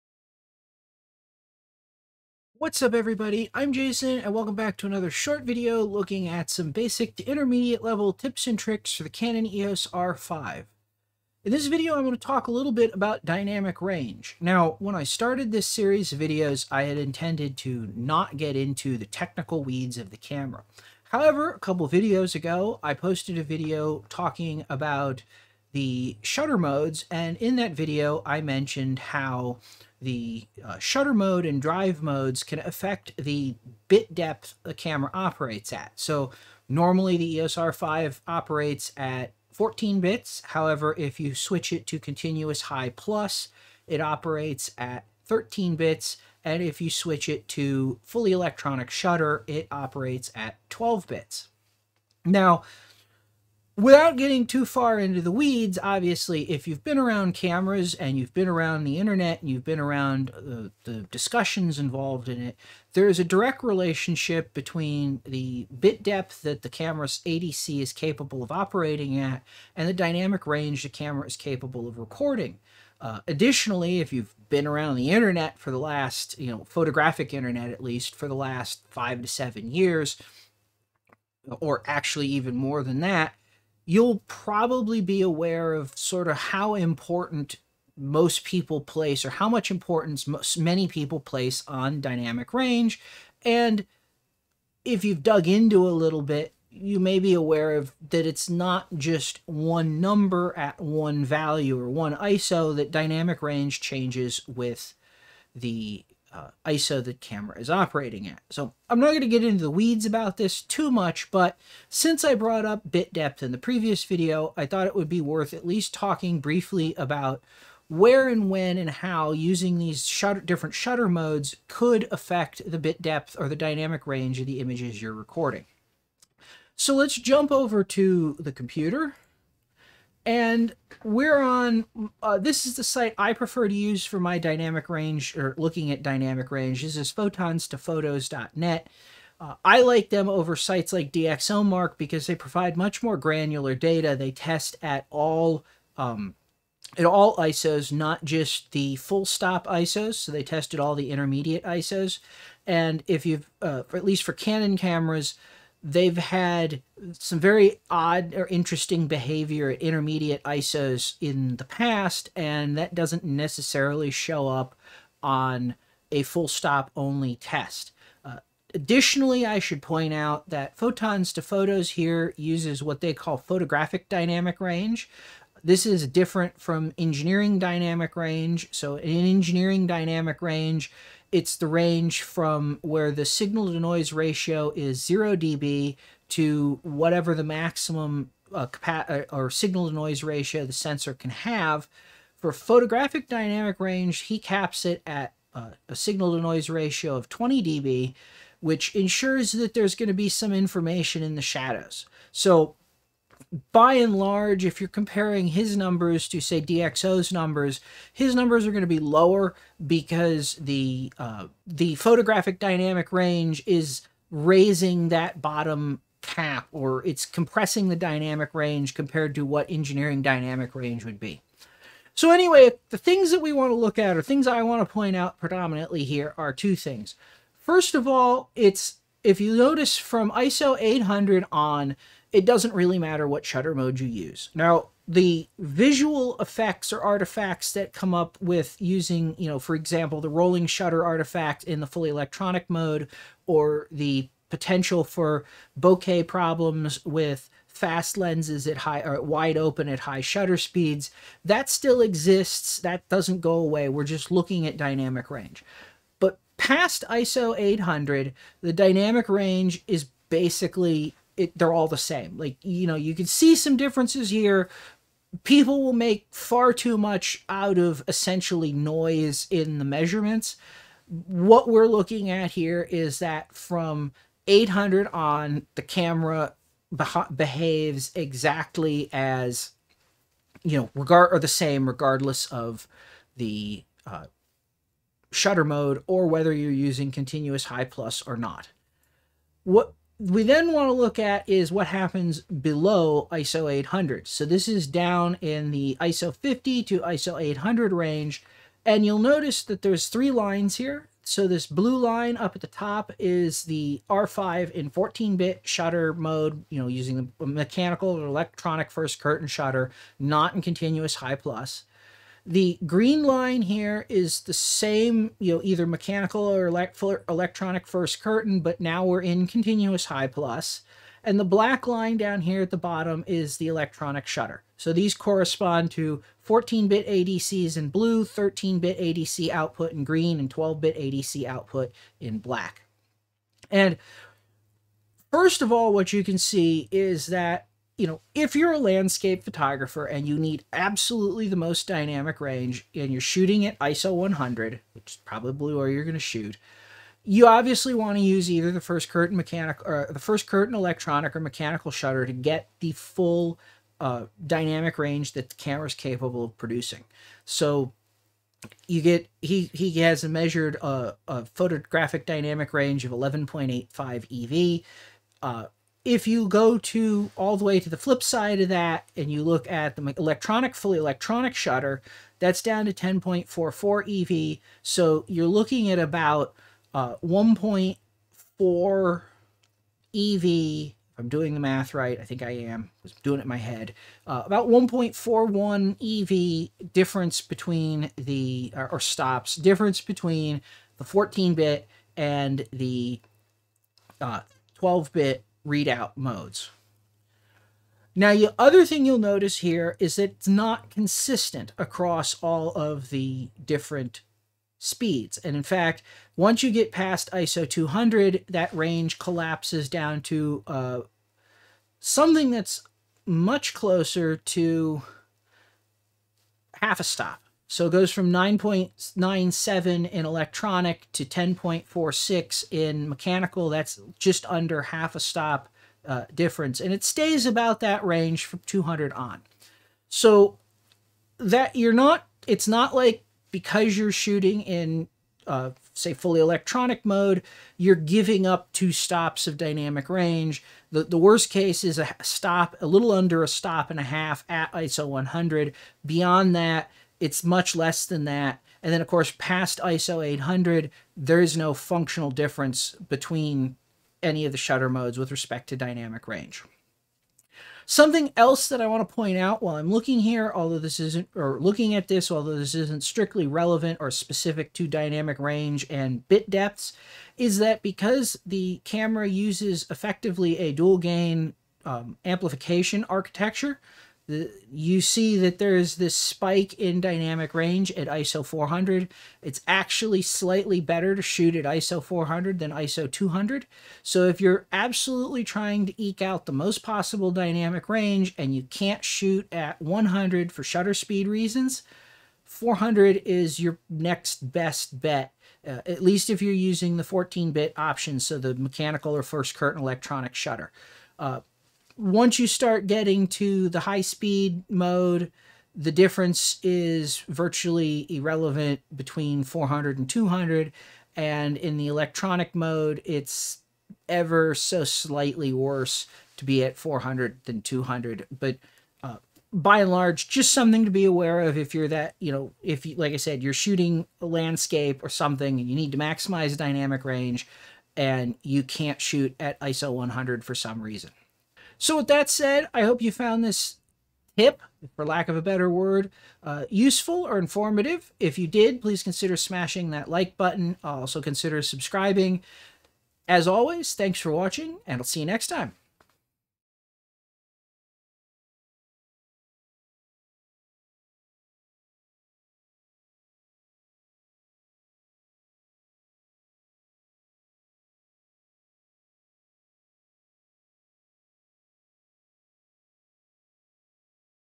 EOS R5 Dynamic Range - EOS R5 Tip 8
In my previous video looking at the shutter modes on the EOS R5, I mentioned that some drive and shutter modes cause the camera to operate at less than the full 14-bit ADC conversion range. Additionally, if you've ever looked into dynamic range in digital systems, you'll know that the bit depth of the ADC directly translates to the available dynamic range. In this video, I'm going to look briefly at the impact of shooting in either 13-bit (using continuous high+) or 12-bit (using the fully electronic shutter) on the image's dynamic range. Spoiler; it's not as much as you might expect.

00:00    Intro

Camera Gear:
Canon EOS 5D mark IV
Canon EF 24-70mm f/2.8L II USM

Audio Gear
Rode NTG2
Zoom F6

Misc
Benro Travel Angle Tripod